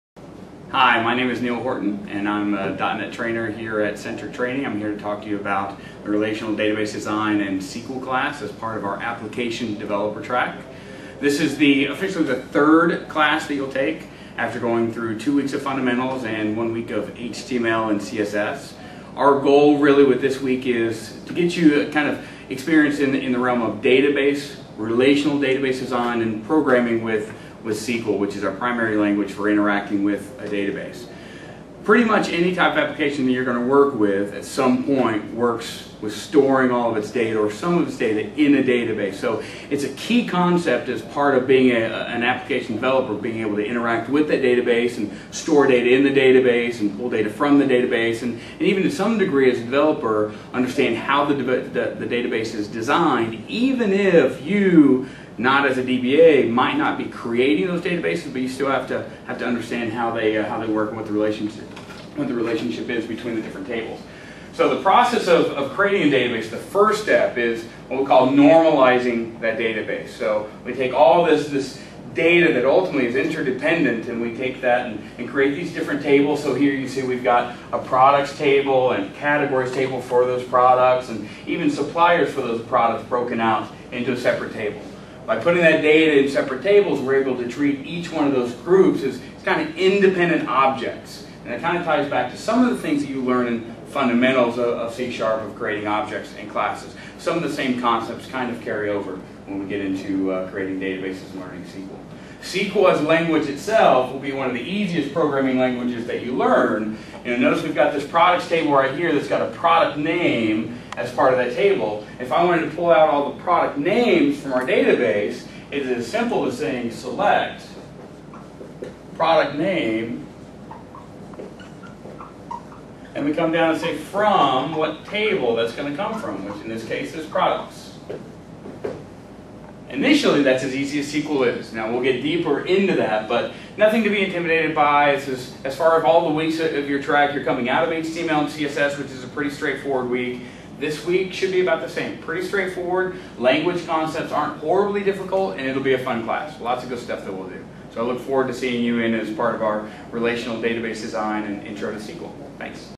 Neill Horton: FRDD/Intro to SQL
Curious about what you'll learn in this class? Instructor Neill Horton shows how learning to organize data fits into the course.